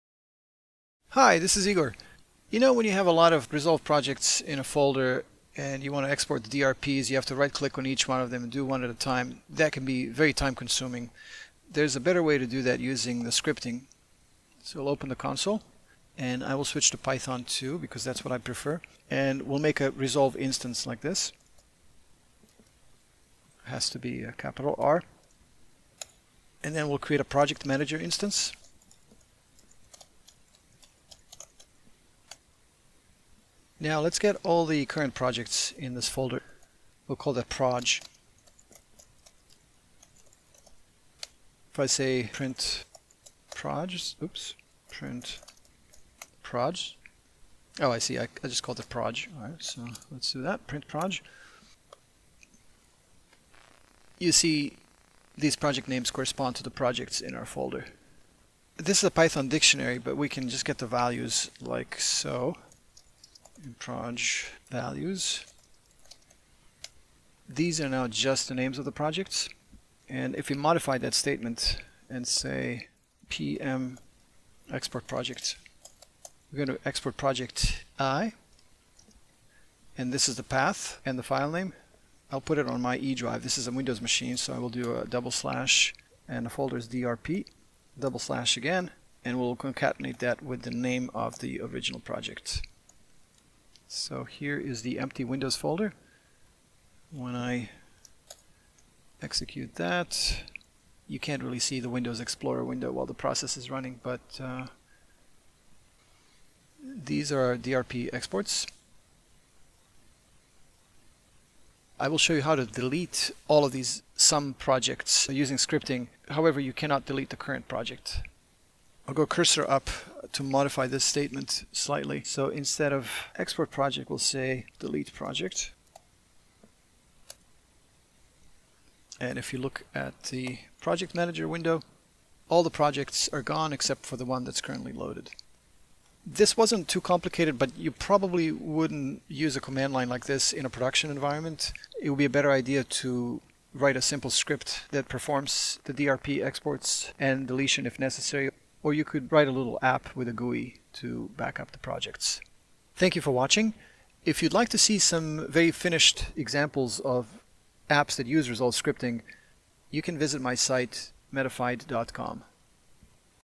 DaVinci Resolve Export Projects Via Scripting
Two new features in the DaVinci Resolve scripting API introduced in V16 allow project export and project deletion. These additions open doors to automated project management like batch DRP exports.

This video demonstrates DRP export and project deletion via the command line. A sample script that can be used as a basis for an automated approach is available